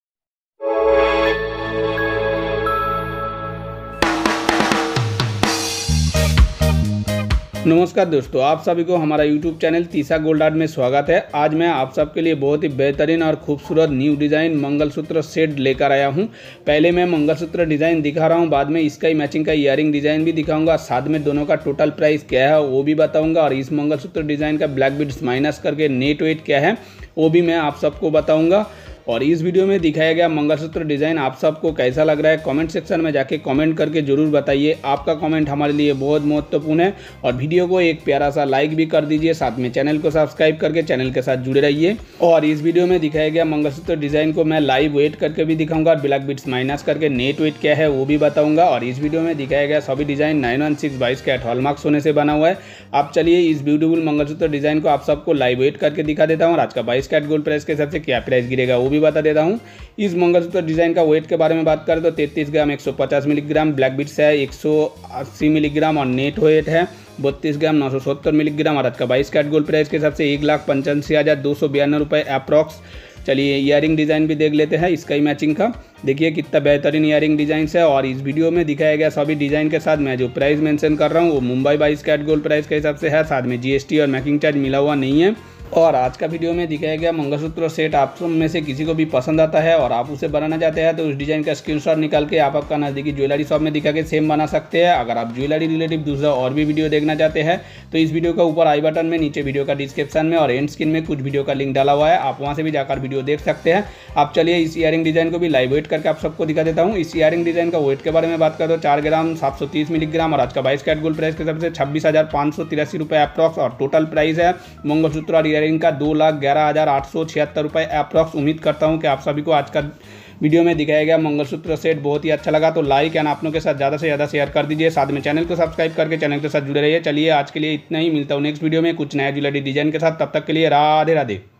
New Gold मंगलसूत्र Set Design With Price || Mangalsutra Designs
Hi , I am Tarun Santra and you are watching trisha gold art
Today I wil show you some New Gold मंगलसूत्र Set Design With Price || Mangalsutra Designs. All the gold mangalsutra designs shown in today's video are made of 916, 22 Carat hallmarked gold.And in today's video I am also showing live weight one by one all gold mangalsutra designs shown. Along with this, I am also telling what is the price of all gold mangalsutra designs according to today's 22 carat gold price.And the price that I am telling in this video along with all the gold mangalsutra designs is according to Mumbai 22 carat gold price.Also GST and making charges are not included.please watch previous video of

Tarun santra
For :- Anjangaon Maharashtra ( India )